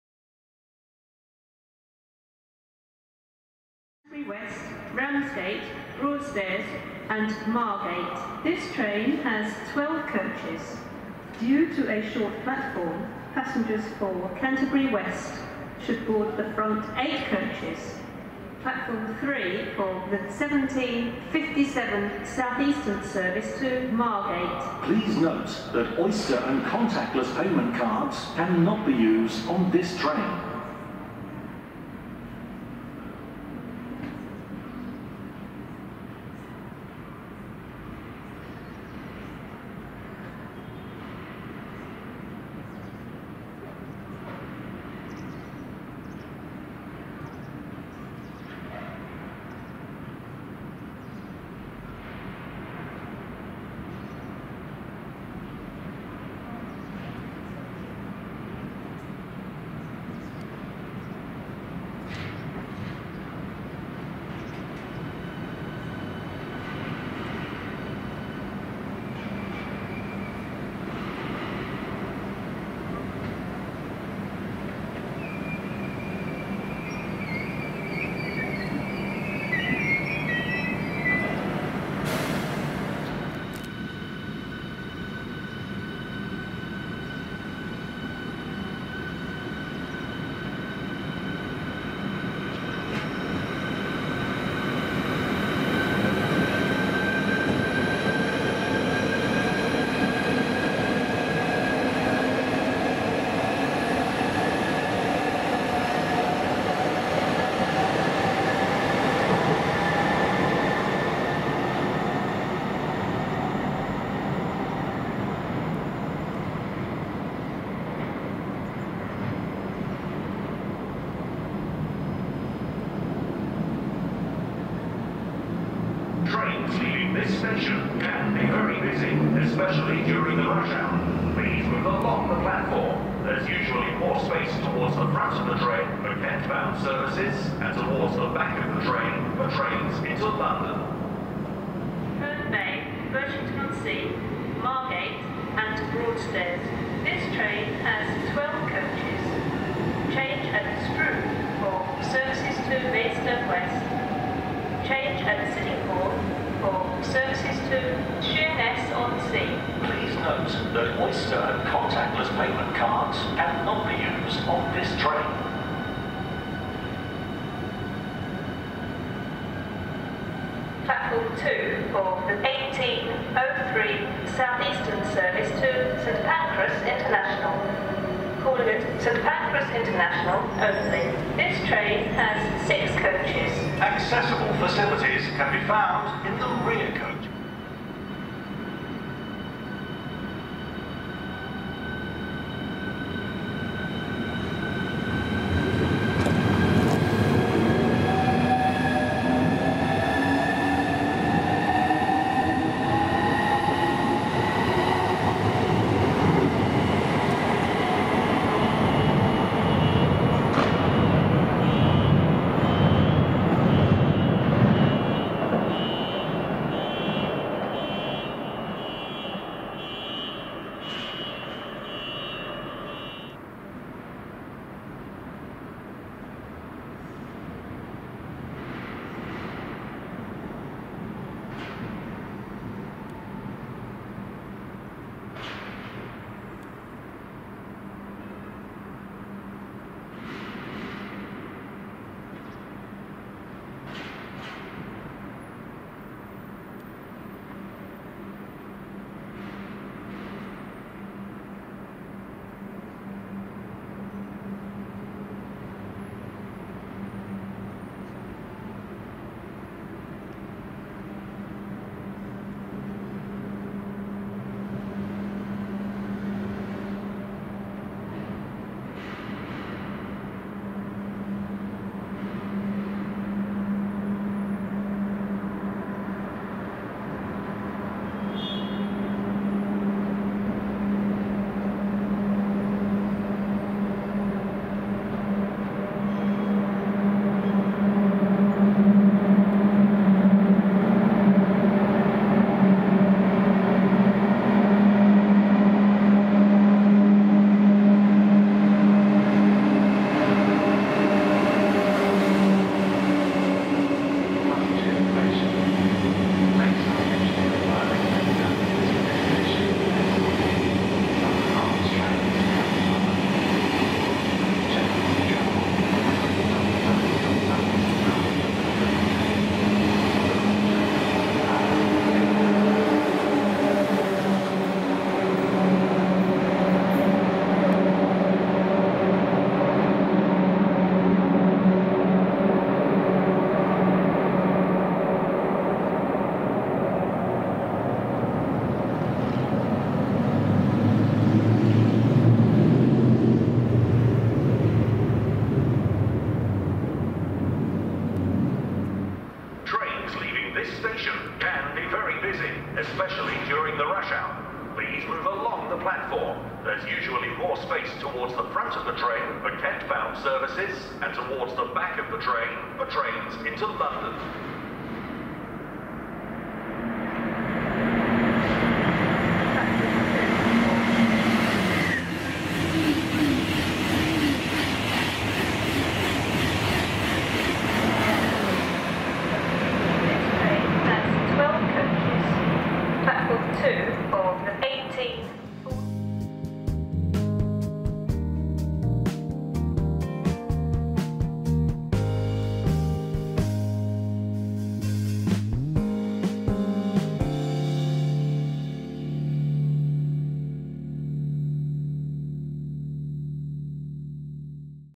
Trains at Stratford Intl
Includes:
Class 373
Class 395

TOCs Included:
Eurostar
Southeastern